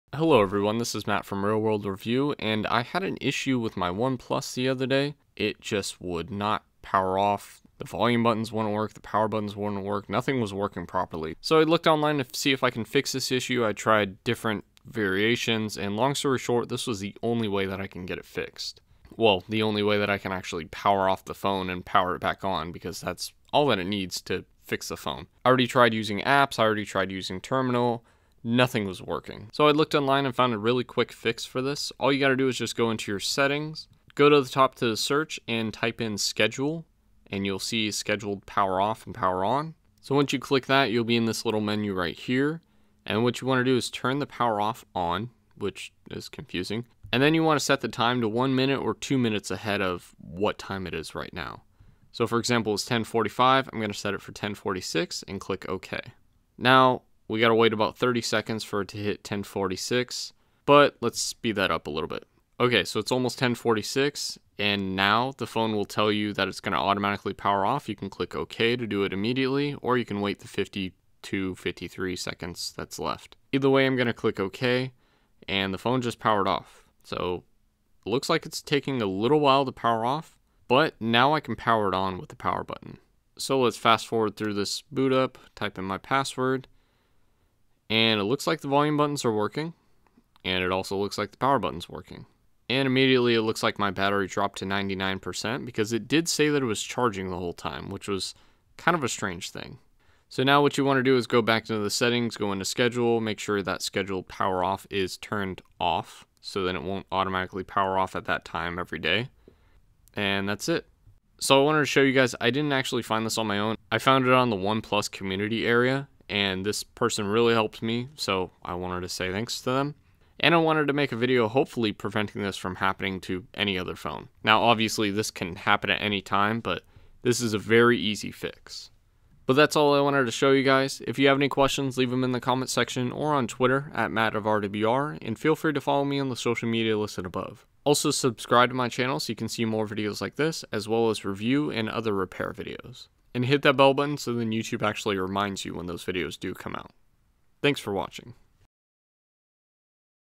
How To Restart Androids with Stuck Buttons (Force Restart on OnePlus 6T and other Android Phones)
The other day my OnePlus 6T stopped accepting button presses for my Power Buttons and Volume Buttons. There weren't any apps that would restart my phone, except for the method in the video. It would also tell me that the battery was not only charging but it was at 100% for over an hour. Regardless, I was able to fix it and this works on most Android phones with 6.0 or higher.
This phone is the OnePlus 6T on Android 9.0 Pie.

Check out the RWR Discord!

This video may or may not fix the broken buttons, but it might fix the buttons if they suffer some software related issues.

What else should I fix? Want more common issue videos? Let me know on here or on Twitter